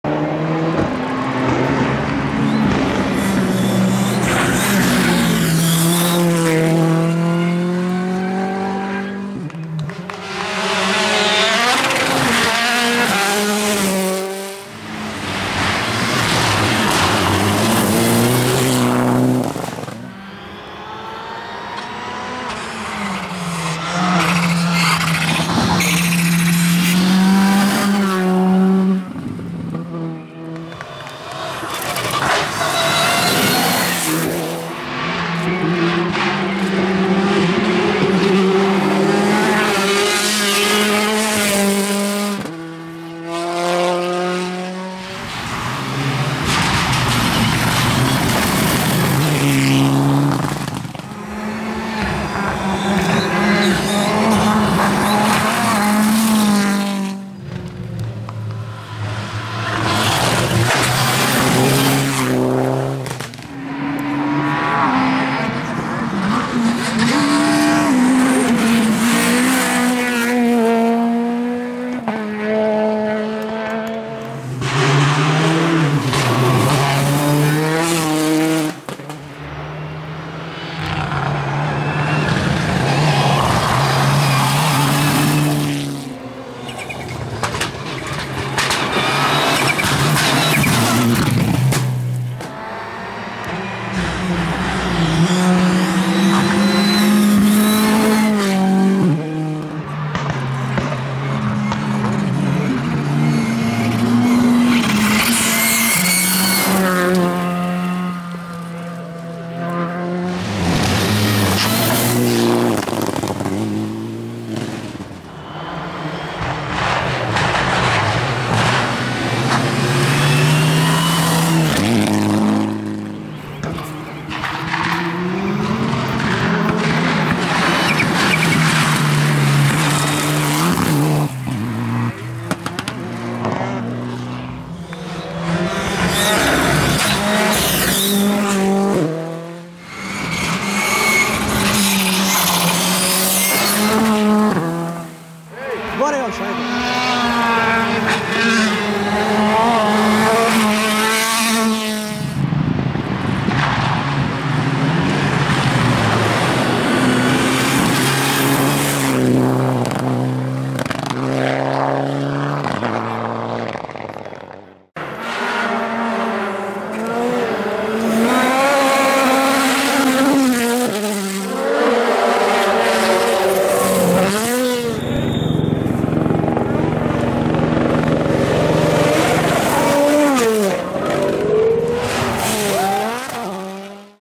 Rally Kumrovec 2017 - Jump, Pure Sound [HD]
Pure Engine Sound!! Victory for Niki Mayr-Melnhof (Ford Fiesta R5) - biggest Jump

Du möchtest die Aufnahmen von dir bei diesem Event haben?

You want to have the scenes from you at this event?